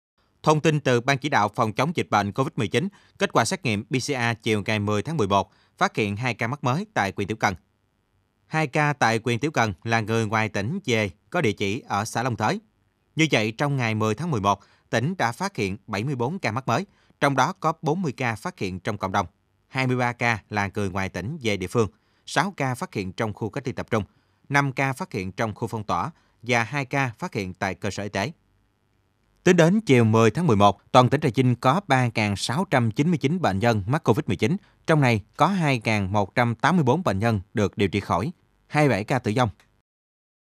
Chiều 10/11/2021, tỉnh Trà Vinh phát hiện 02 ca mắc COVID-19
Kết quả xét nghiệm PCR chiều ngày 10/11/2021, tỉnh Trà Vinh phát hiện 02 ca mắc COVID-19 mới tại huyện Tiểu Cần, là người ngoài tỉnh về địa phương, được cách ly và khẳng định tại BVĐK tỉnh.
Như vậy, trong ngày 10/11/2021 tỉnh Trà Vinh đã phát hiện 74 ca mắc mới (38 ca phát hiện trong cộng đồng, 23 là người ngoài tỉnh về địa phương, 06 ca phát hiện trong khu CLTT, 05 ca phát hiện trong khu phong tỏa và 02 ca phát hiện tại CSYT).
Trong 74 ca mắc COVID-19 ngày hôm nay có:
+ 13/74 ca tiêm 02 mũi vắc xin
+15/74 ca tiêm 01 mũi vắc xin